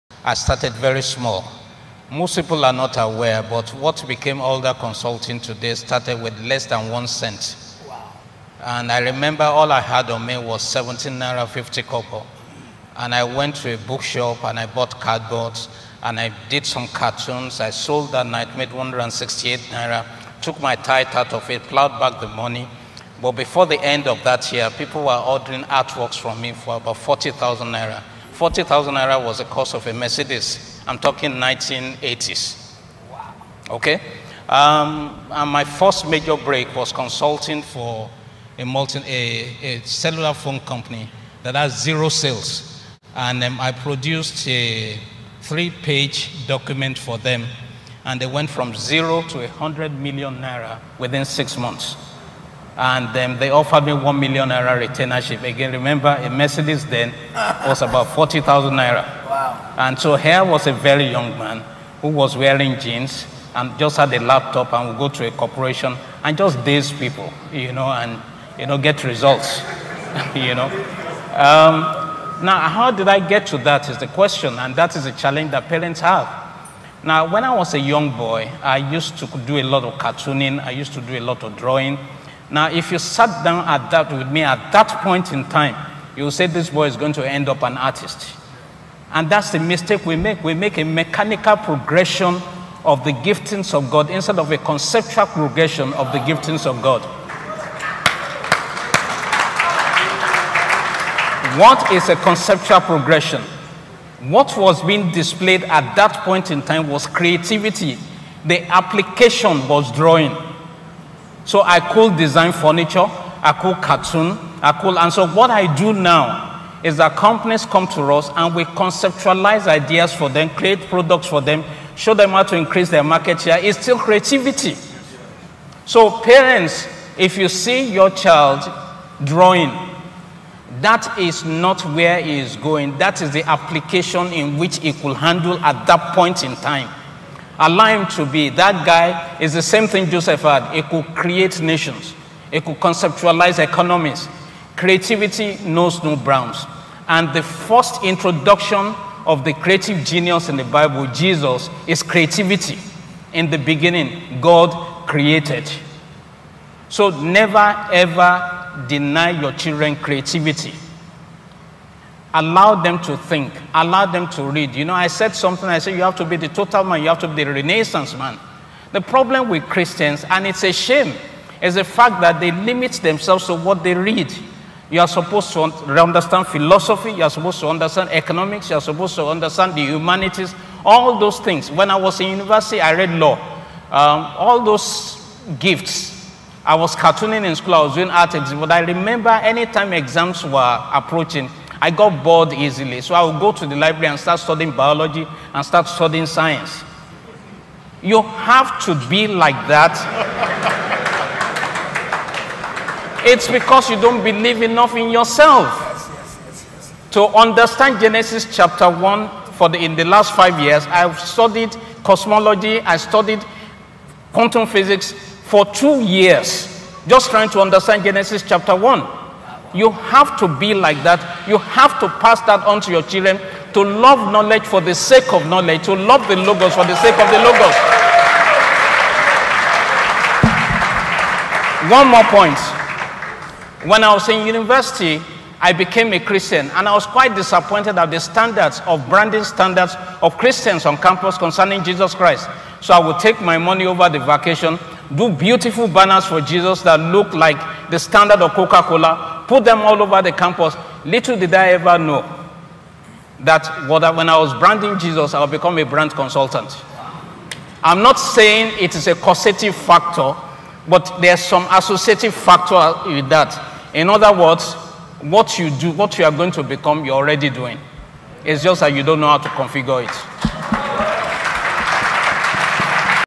HOW LEKE ALDER STARTED HIS BUSINESS WITH ONLY #17.50K AND BECAME VERY WAELTHY
This video explains how Leke Alder began his business career with less than 1 cent and today became very wealthy. His is the Principal of Alder Consulting.